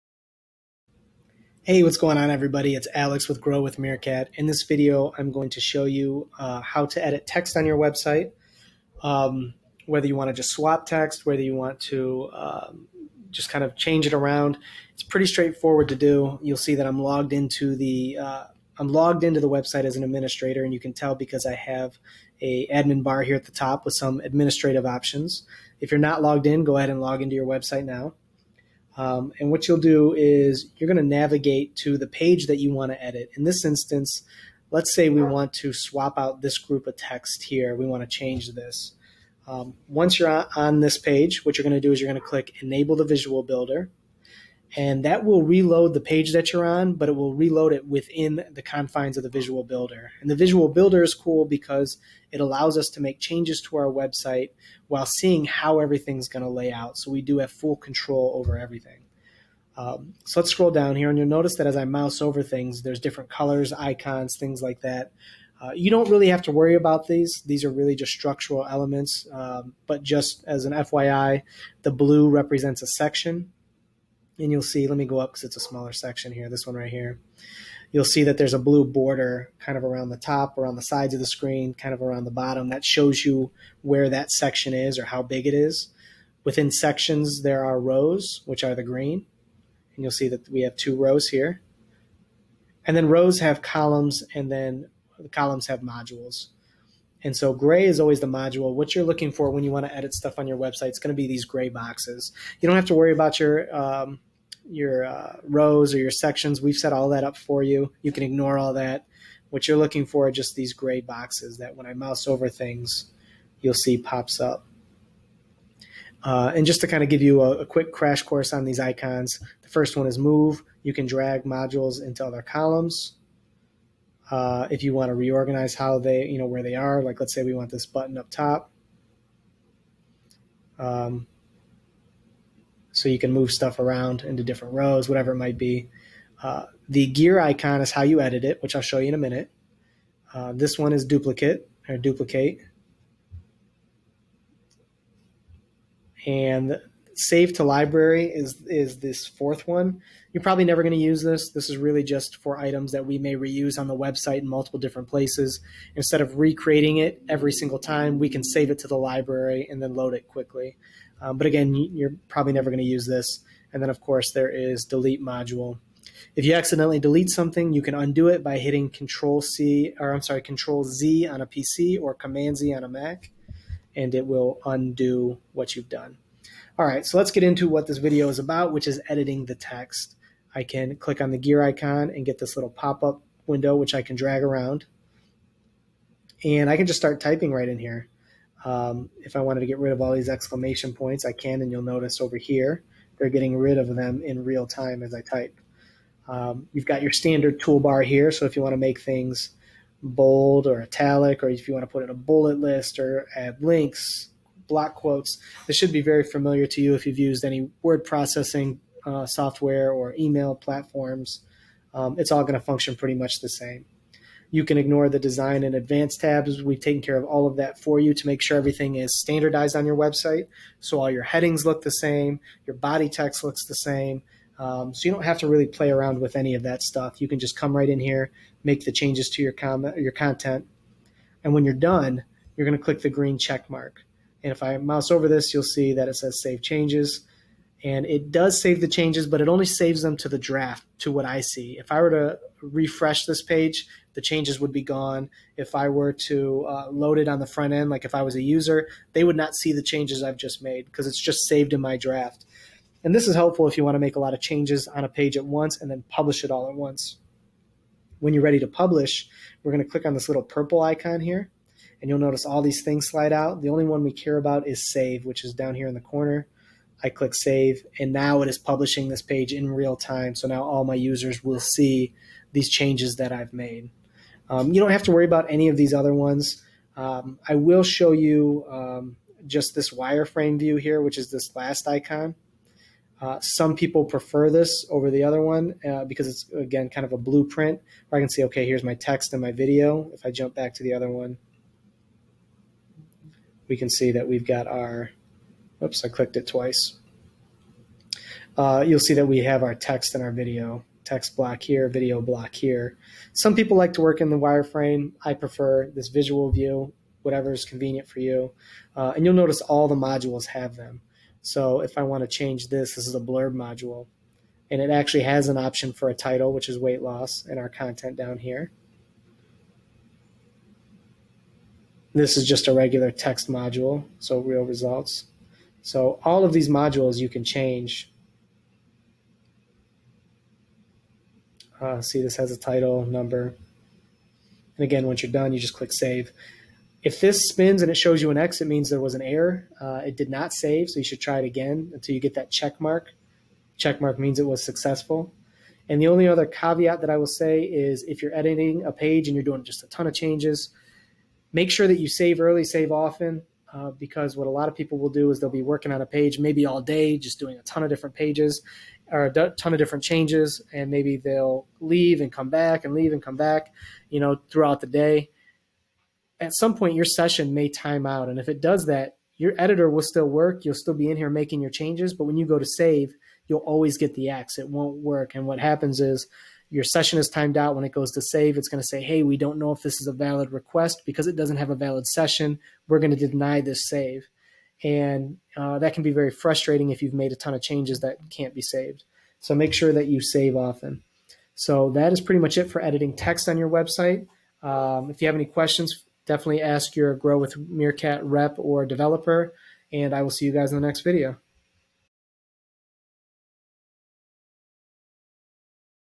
Editing Text on Website Pages
A short video demonstrating how to edit text on websites that are built by Grow with Meerkat.